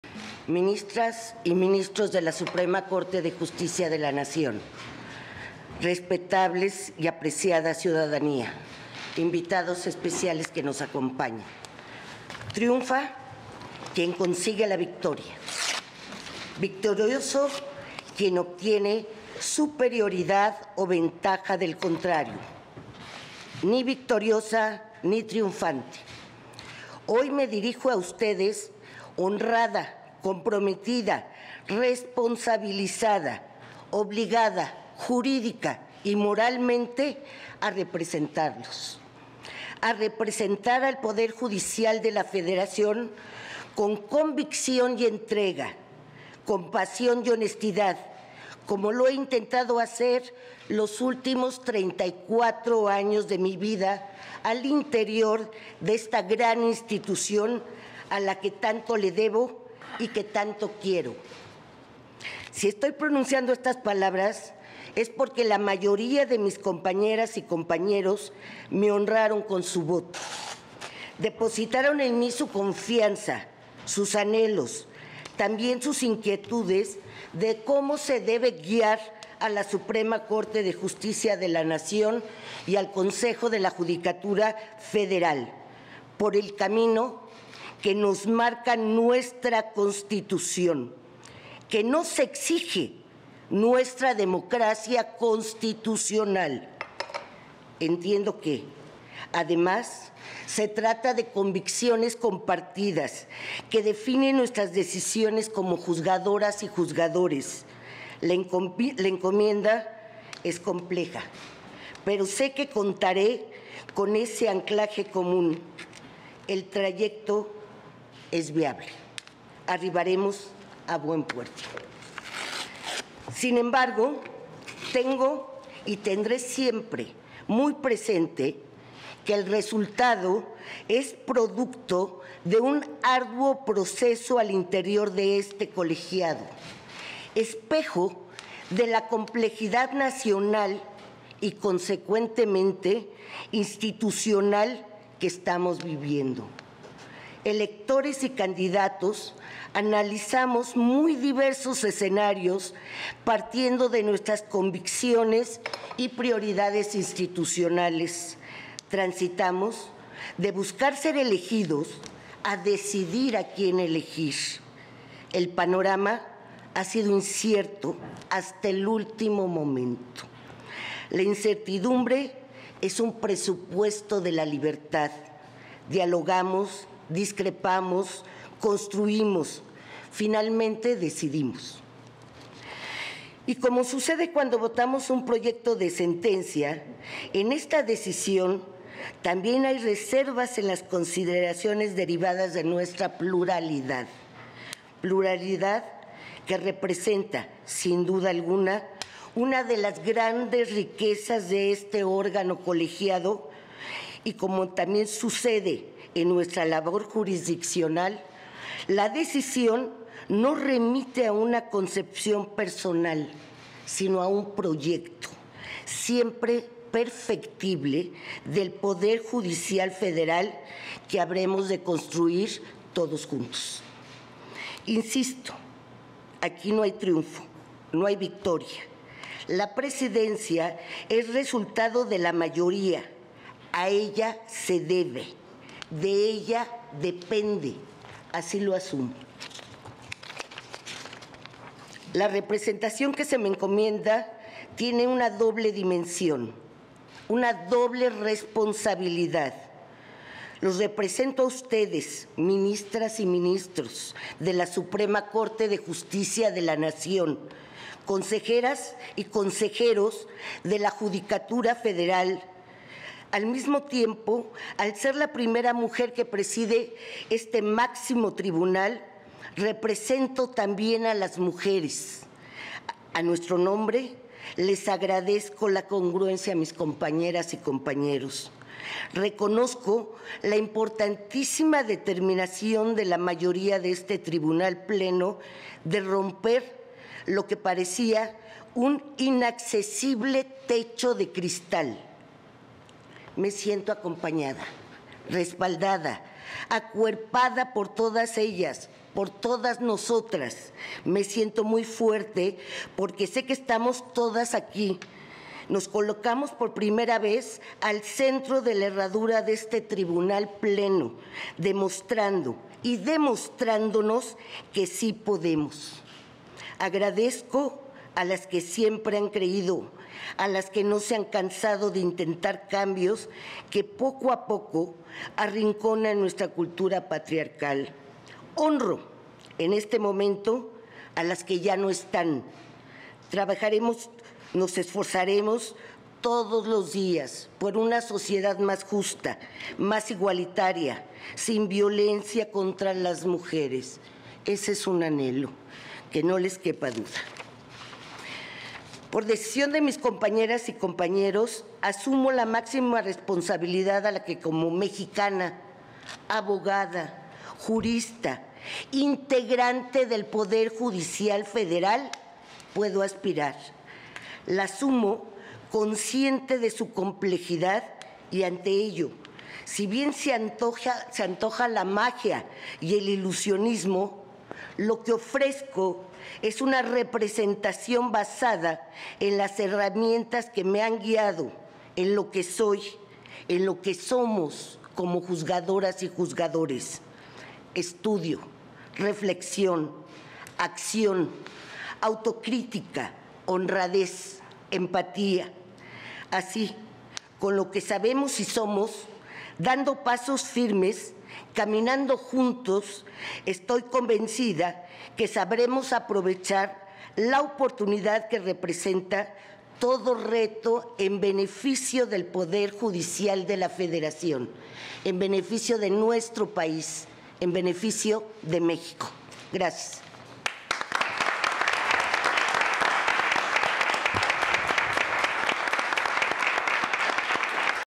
Palabras de la Ministra Norma Lucía Piña Hernández al rendir protesta como Presidenta de SCJN y CJF
Ministras y ministros de la Suprema Corte de Justicia de la Nación.
Respetable y apreciada ciudadanía.
Invitados especiales que nos acompañan.
Triunfa quien consigue la victoria. Victorioso quien obtiene superioridad o ventaja
del contrario. Ni victoriosa, ni triunfante.
Hoy me dirijo a ustedes honrada, comprometida, responsabilizada, obligada, jurídica, y moralmente a representarlos. A representar al Poder Judicial de la Federación con convicción y entrega, con pasión y honestidad. Como lo he intentado hacer los últimos 34 años de mi vida al interior de esta gran institución a la que tanto le debo y que tanto quiero.
Si estoy pronunciando estas palabras, es porque la mayoría de mis compañeras y compañeros me honraron con su voto. Depositaron en mí su confianza, sus anhelos, también sus inquietudes de cómo se debe guiar a la Suprema Corte de Justicia de la Nación y al Consejo de la Judicatura Federal, por el camino que nos marca nuestra Constitución; que nos exige nuestra democracia constitucional.
Entiendo que, además, se trata de convicciones compartidas. Que definen nuestras decisiones como juzgadoras y juzgadores. La encomienda es compleja, pero sé que contaré con ese anclaje común. El trayecto es viable. Arribaremos a buen puerto.
Sin embargo, tengo y tendré siempre muy presente que el resultado es producto de un arduo proceso al interior de este colegiado, espejo de la complejidad nacional y consecuentemente, institucional que estamos viviendo; electores y candidatos analizamos muy diversos escenarios partiendo de nuestras convicciones y
prioridades institucionales; transitamos de buscar ser elegidos a decidir a quién elegir; el panorama ha sido incierto hasta el último momento; la incertidumbre es un presupuesto de la libertad, dialogamos, discrepamos, construimos, finalmente, decidimos.
Y como sucede cuando votamos un proyecto de sentencia, en esta decisión también hay reservas en las consideraciones derivadas de nuestra pluralidad, pluralidad que representa, sin duda alguna, una de las grandes riquezas de este órgano colegiado, y como también sucede en nuestra labor jurisdiccional, la decisión no remite a una concepción personal, sino a un proyecto, siempre perfectible del Poder Judicial
Federal que habremos de construir todos juntos.
Insisto, aquí no hay triunfo, no hay victoria, la Presidencia es resultado de la mayoría, a ella se debe, de ella depende, así lo asumo.
La representación que se me encomienda tiene una doble dimensión, una doble responsabilidad, los representa a ustedes ministras y ministros de la Suprema Corte de Justicia de la Nación, consejeras y consejeros de la Judicatura Federal, al mismo tiempo, al ser la primera mujer que preside este Máximo Tribunal, represento también a las mujeres, a nuestro nombre les agradezco la congruencia a mis compañeras y compañeros, reconozco la importantísima determinación de la mayoría de este Tribunal Pleno, de romper lo que parecía, un inaccesible techo de cristal, me siento acompañada, respalda, acuerpada por todas ellas, por todas nosotras. Me siento muy fuerte porque sé que estamos todas aquí, nos colocamos por primera vez al centro de la herradura de este Tribunal Pleno demostrando y demostrándonos que sí podemos.
Agradezco a las que siempre han creído, a las que no se han cansado de intentar cambios que poco a poco arrinconan nuestra cultura patriarcal, honro en este momento, a las que ya no están. Trabajaremos, nos esforzaremos todos los días por una sociedad más justa, más igualitaria, sin violencia contra las mujeres, ese un anhelo, que no les quepa duda.
Por decisión de mis compañeras y compañeros asumo la máxima responsabilidad a la que como mexicana, abogada, jurista, integrante del Poder Judicial Federal, puedo aspirar, la asumo consciente de su complejidad, y ante ello, si bien se antoja la magia y el ilusionismo, lo que ofrezco es una representación basada en las herramientas que me han guiado en lo que soy, en lo que somos como juzgadoras y juzgadores; estudio, reflexión, acción, autocrítica; honradez, empatía, así con lo que sabemos y somos, dando pasos firmes, caminando juntos, estoy convencida que sabremos aprovechar la oportunidad que representa todo reto en beneficio del Poder Judicial de la Federación, en beneficio de nuestro país, en beneficio de México.
Gracias.